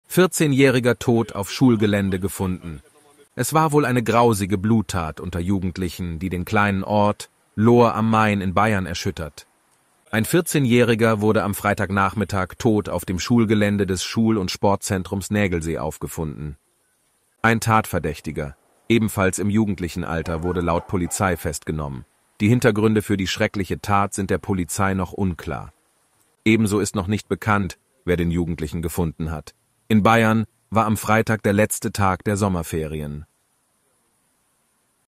08.09.2023 - Junge (14) tot auf Schulgelände in Lohr am Main gefunden
Am Freitagnachmittag (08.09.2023) erschüttert eine Bluttat die kleine Stadt Lohr am Main im Kreis Main-Spessart: Ein Jugendlicher hat nach ersten Informationen einen 14-Jährigen auf dem Pausenhof am dortigen Schulzentrum angegriffen und schwerst verletzt. Der Junge starb noch vor Ort.

Polizeibeamte konnten den Tatverdächtigen "am Wohnort" festnehmen. Wie ein Sprecher der Polizei bestätigte, wurde die Leiche des Jugendlichen gegen 16:30 Uhr auf dem Gelände des Schulzentrums Nägelsee gefunden. "Der genaue Tatablauf ist Gegenstand der Ermittlungen", so der Pressesprecher der Polizei. Die Kriminalpolizei Würzburg ist vor Ort und hat die Ermittlungen übernommen.